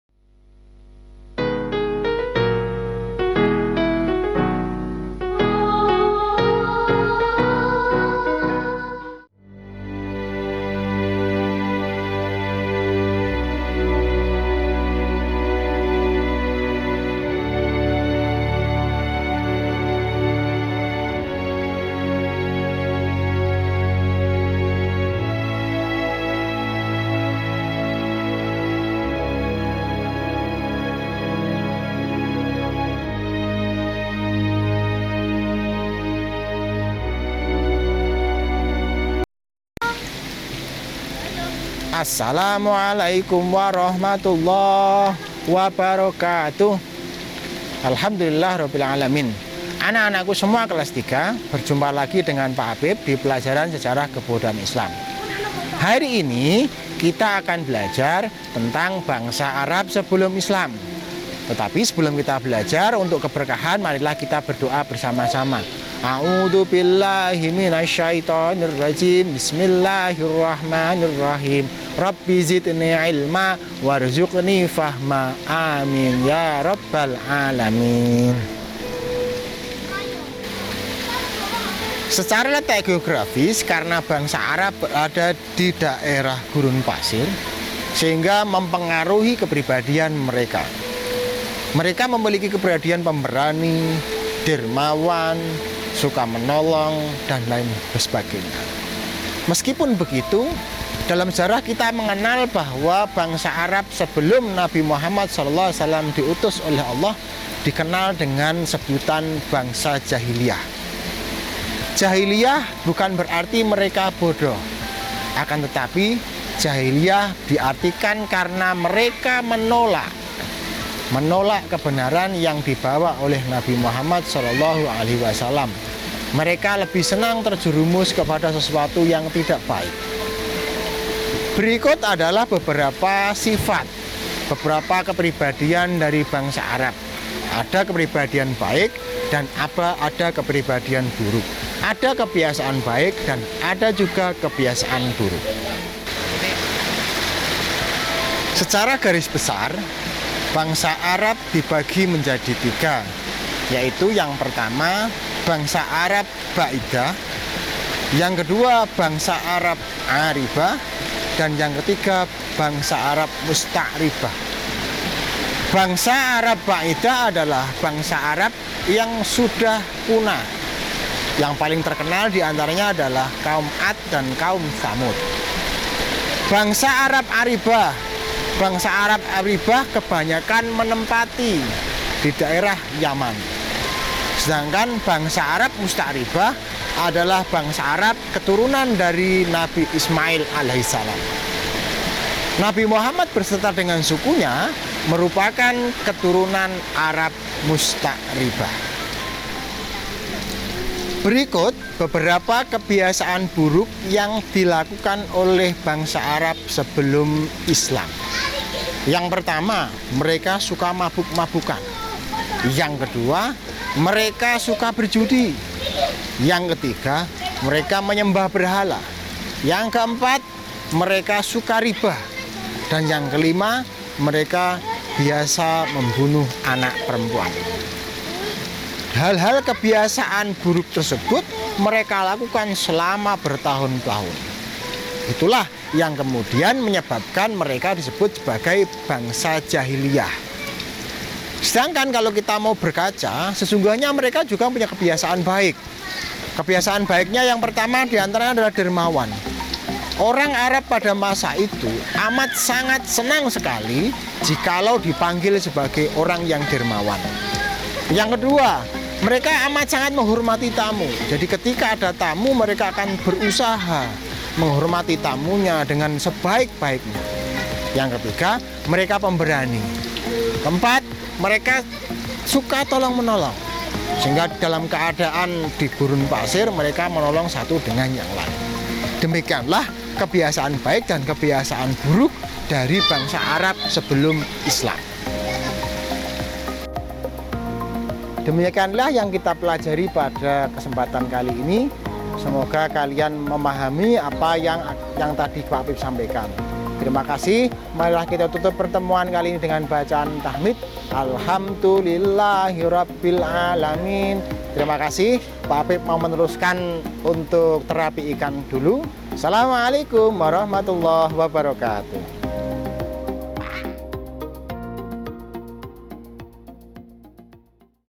SKI KELAS 3 BAB 1 KEBIASAAN BAIK DAN KEBIASAAN BURUK BANGSA ARAB SEBELUM ISLAM
Keadaan Sosial Masyarakat Arab sebelum Islam
- Kebiasaan baik
- Kebiasaan buruk
Materi sesuai dengan KMA 183 Tahun 2019
kurikulum yang terbaru